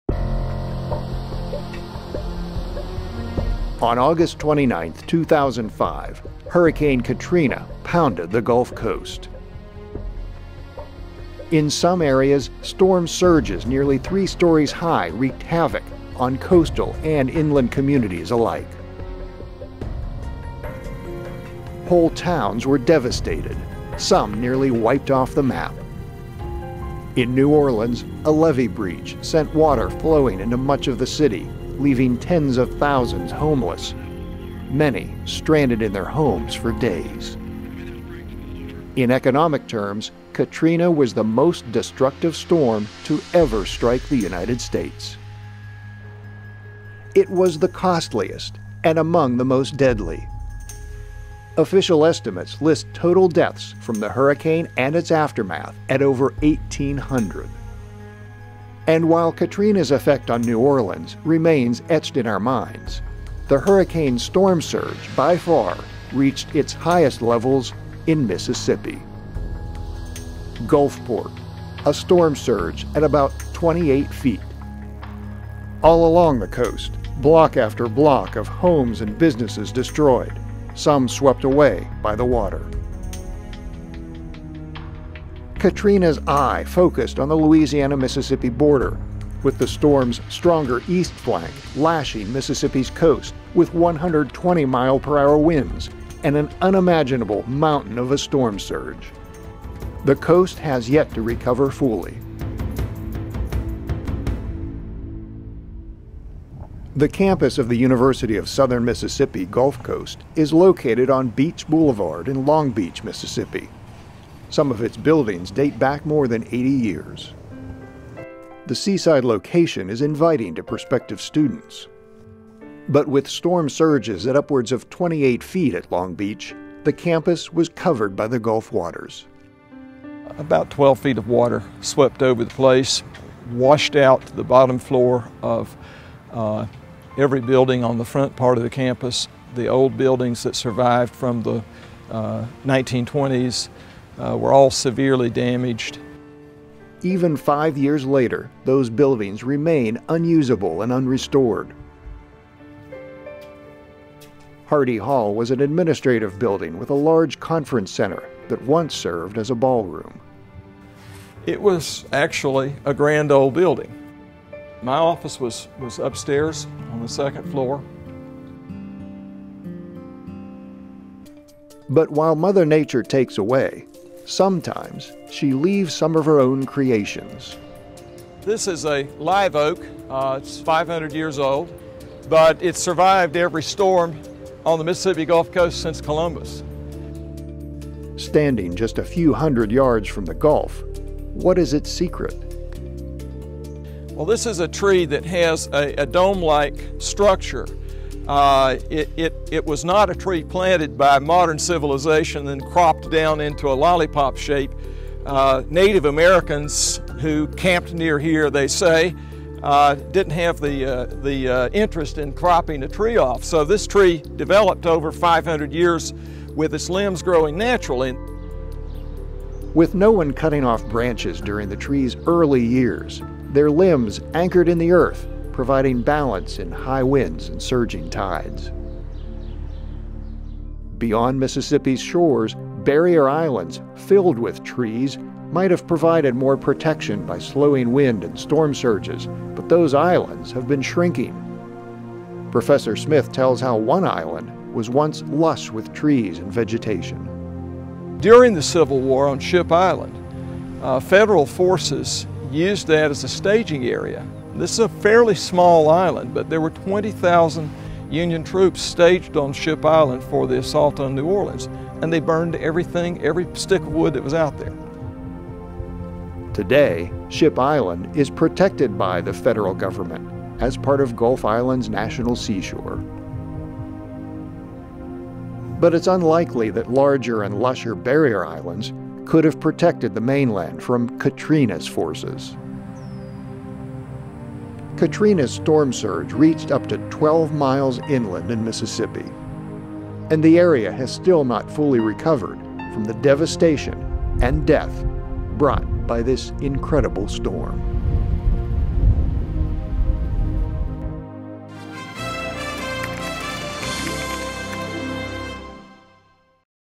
5 Years After Katrina, Ruins - And a Natural Wonder - Remain | National Geographic
Hurricane Katrina devastated the U.S. Gulf Coast communities five years ago, leaving behind ruins that persist to this day. But at least one of Mother Nature's iconic creations survived intact, and still inspires.